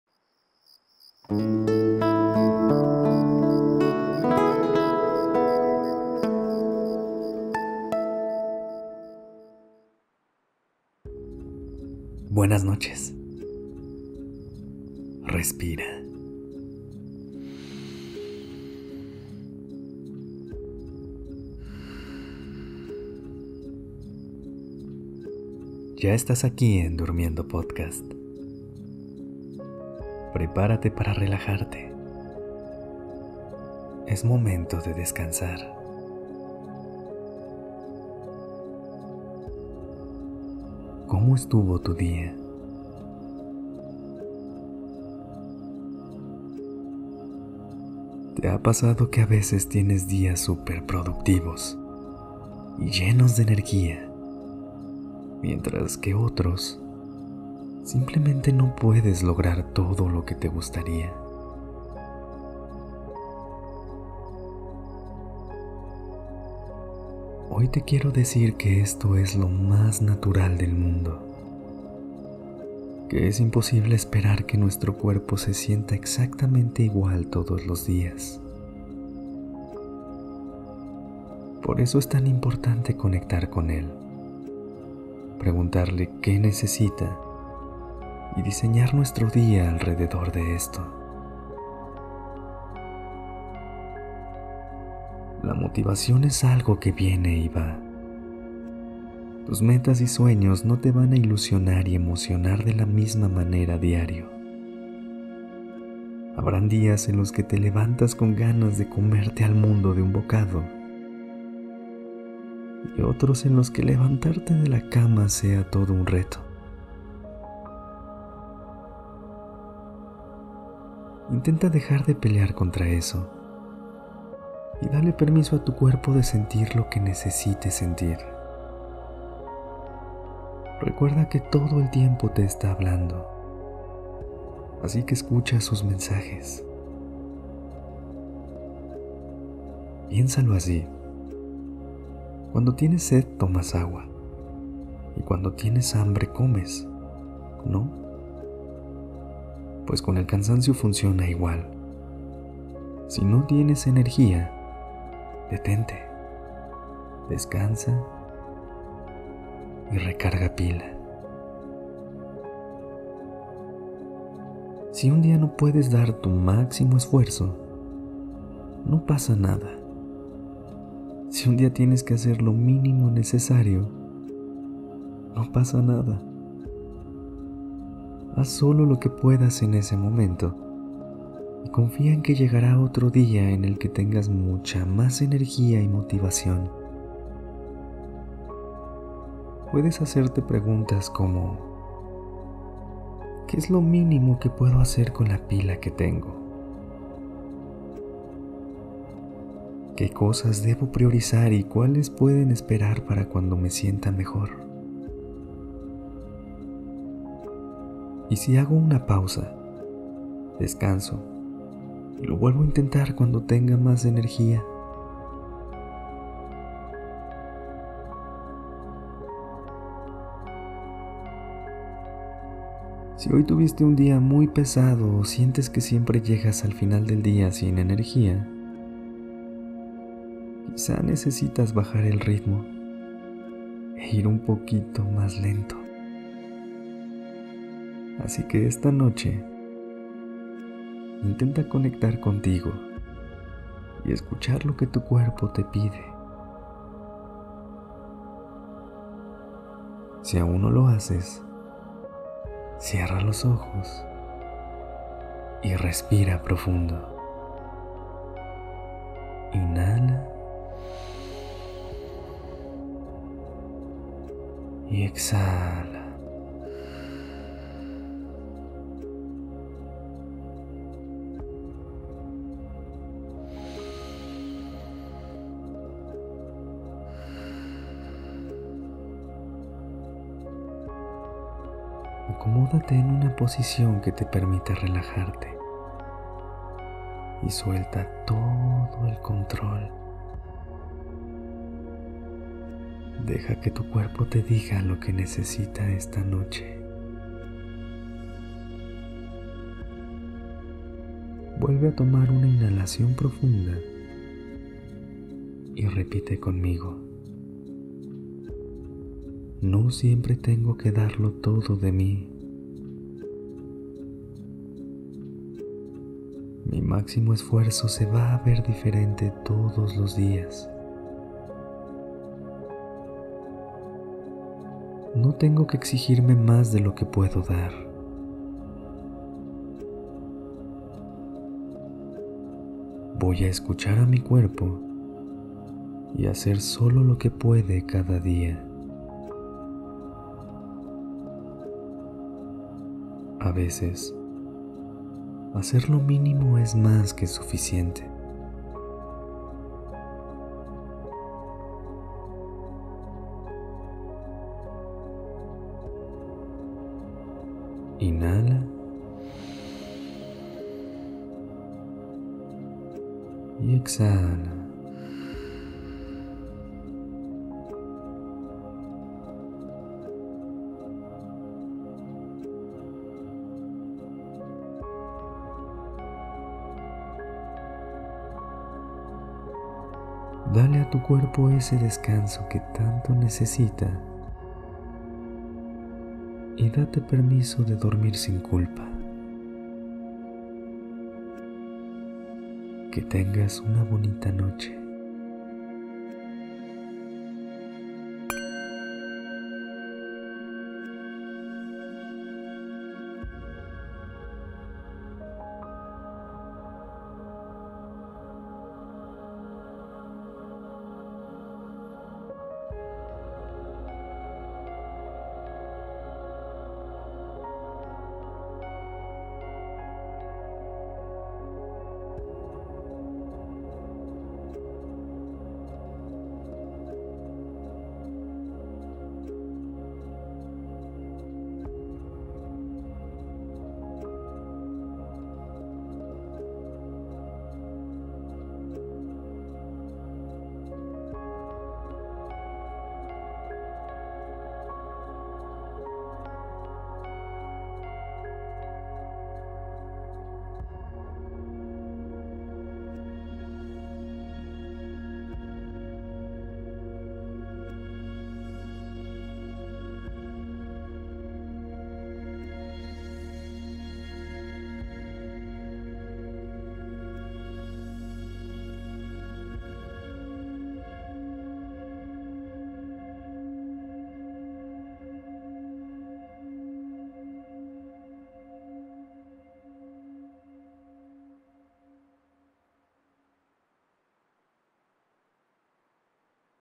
¿Qué Hacer en los Días en los que NO TENGO MOTIVACIÓN? - Episodio 31 | Durmiendo Podcast
Hoy te compartimos algunas afirmaciones para que empieces a confiar en ti y en tu capacidad de lograr todas las cosas que te propones.

En este episodio hablamos de:
-  Entender que el universo está a tu favor
-  Lo que buscas, también te está buscando a ti
-  Eres capaz de todo, solo tienes que creértela
☀️🌙 ¿Qué son DespertandoPodcast y DurmiendoPodcast?
Sigue a nuestras Creadoras:
💕 Lety Sahagún